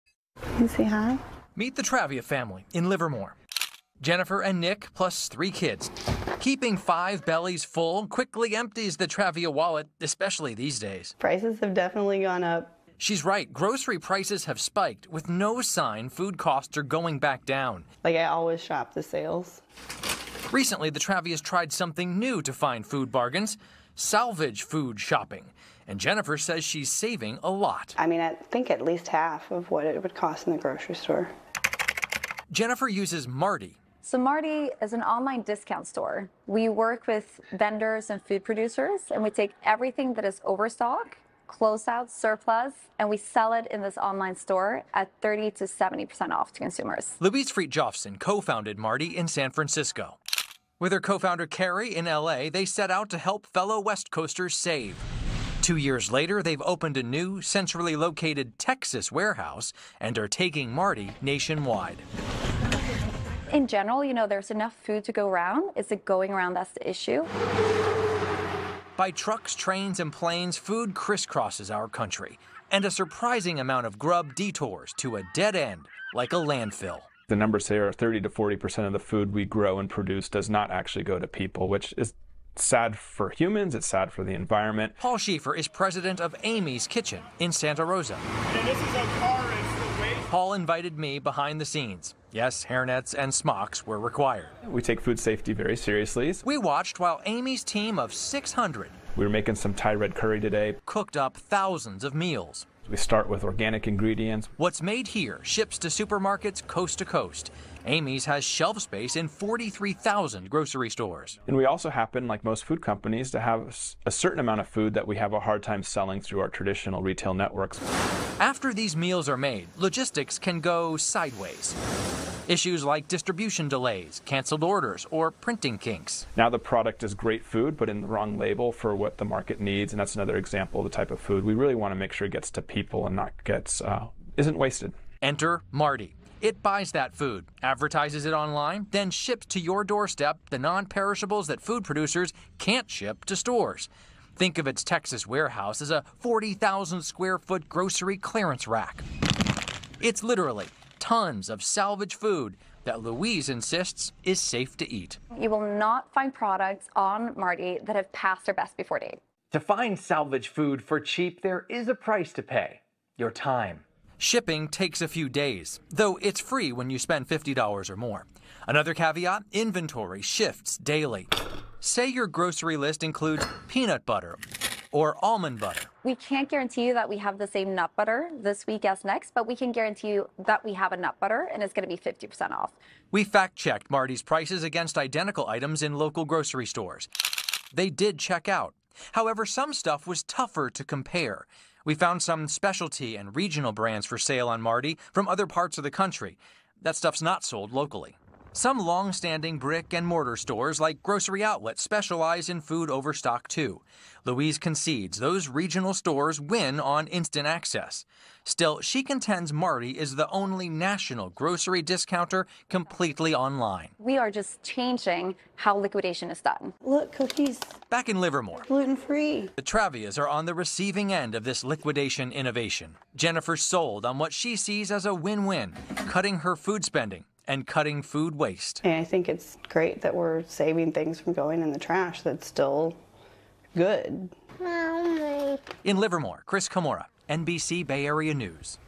SF-born app offers up to 70% off groceries. We price checked 'em
We Googled “How can you save money on groceries?”
And we saw a long list of supposed food hacks and cheat codes.
…sales cycles, coupon swaps, rewards strategies, and such.

TikTokers and YouTubers have pro tips and hacks, too.
But, there’s another -- maybe easier route -- to save as much as 70% on food: Salvage food.

What is salvage food? Basically, it’s overstock.
Some regional supermarkets (a.k.a liquidators) sell overstock. But their stores aren’t everywhere.

So now, a San Francisco-born company is offering new nationwide access to really cheap food. It was, at first, the founders’ California passion project. Today, Martie is going national  -- shipping to all 50 states and feeding tens of thousands of cost-conscious families. Martie says it sells food for 30% to 70% less than supermarkets.

Oh, really?
We wanted to check out this online bargain hunt. And fact check that 70% off claim.
Here’s some of what we wanted to know:
+Where does salvage food come from?
+Why isn’t salvage food selling in regular grocery stores?
+Is salvage food “expired?” Or past its “best by” date?
+Are salvage food discounts really as deep as they advertise?
+Who’s eating salvage food?

We made four stops:
1.) We met with Martie’s co-founder
2.) We visited Amy’s Kitchen in Santa Rosa -- to take a peek behind the scenes of the food supply chain (yes, Chris wore a hairnet)
3.) We talked with an East Bay family that uses Martie
4.) We went shopping -- and compared Martie’s prices against the grocery store

Join us on this journey. The next four minutes might save you hundreds of dollars a year.